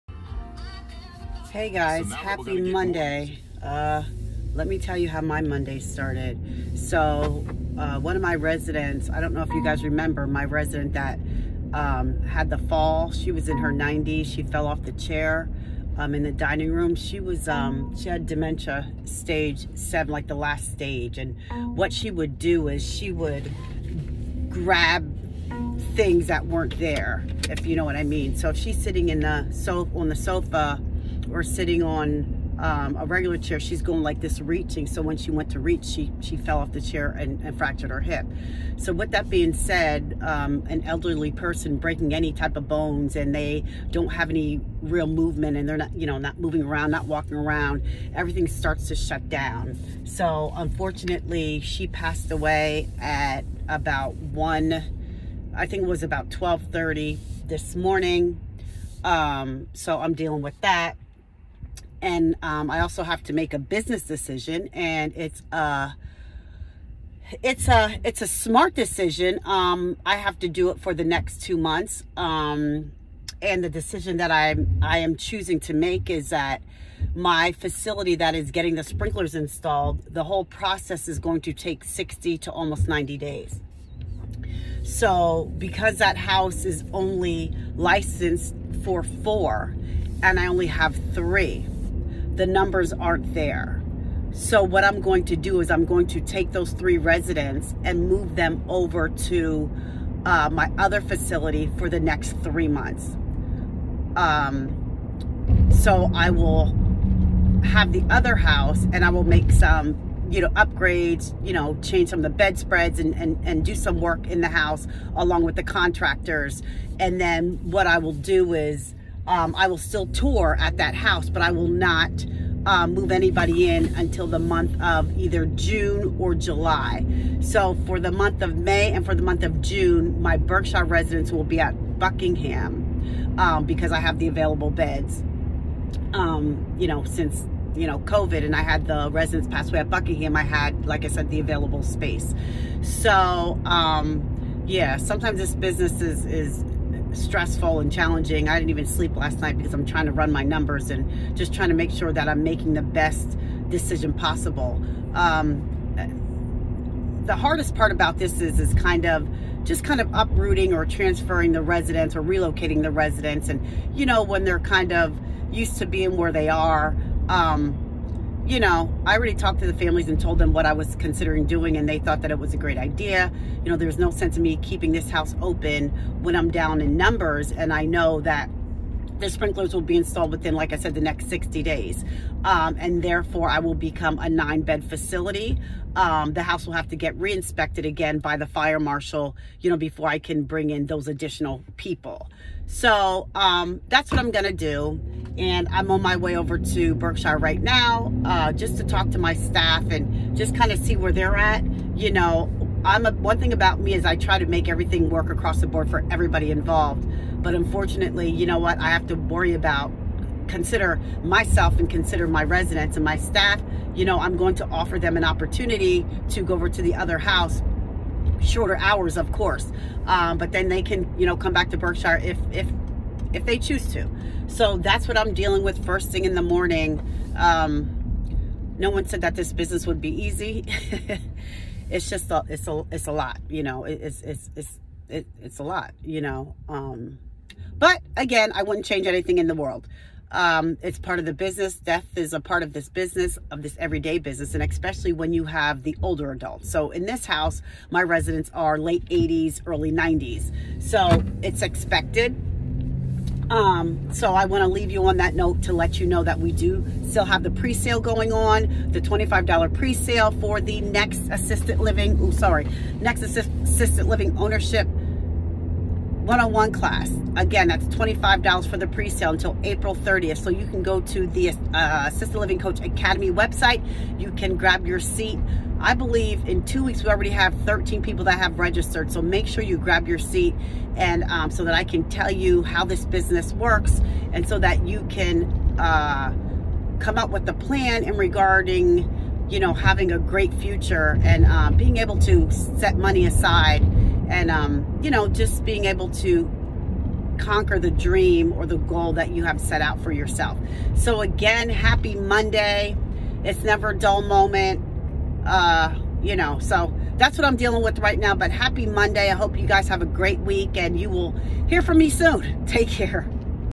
Rollercoaster Ride
This business has many emotions, it makes you feel as though you are on a roller coaster ride!

Making decisions that are sometimes difficult but at the same time a smart business decision.